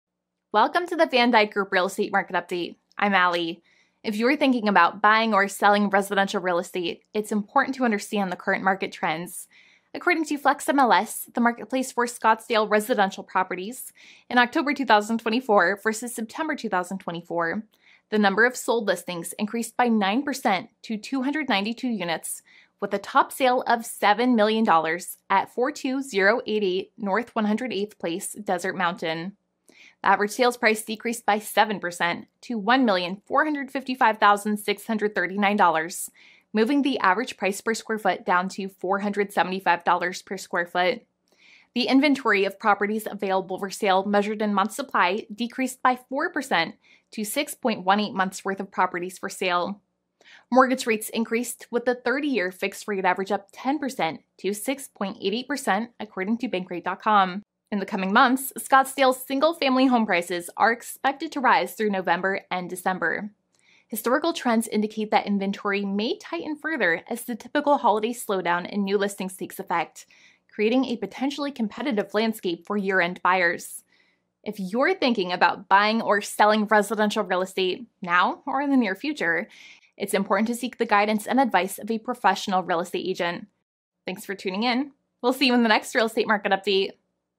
Real Estate Market Update | Scottsdale | October 2024 vs September 2024
Scottsdale
Average Sales Price: September 2024 – $1,562,534 vs October 2024 – $1,455,639 = 7% decrease
Average Days On Market: September 2024 – 88 vs October 2024 – 78 = 11% decrease
Average Sale Price Per Square Foot: September 2024 – $482 vs October 2024 – $475 = 1% decrease
Sale to List Price Ratio: September 2024 – 96.48% vs October 2024 – 96.49% = 0% No Change
Number of Sold Listings: September 2024 – 269 vs October 2024 – 292 = 9% increase
Months Supply of Inventory: September 2024 – 6.45 vs October 2024 – 6.18 = 4% decrease
Mortgage Rate: September 2024 – 6.26% vs October 2024 – 6.88% = 10% increase

Top 10 Sales
$7,000,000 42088 N 108th Place
$5,800,000 8542 E Los Gatos Drive
$5,650,000 9820 E Thompson Peak Pkwy #640
$5,495,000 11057 E Distant Hills Drive
$4,666,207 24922 N 91st Street
$4,600,000 8014 E Sharon Drive
$4,500,000 9408 E Covey Trail
$4,350,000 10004 E Cholla Street
$4,350,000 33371 N Vanishing Trail
$4,325,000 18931 N 97th Place

Grant Van Dyke, Associate Broker
Van Dyke Group | The Griffin
VanDykeFeaturedProperties.com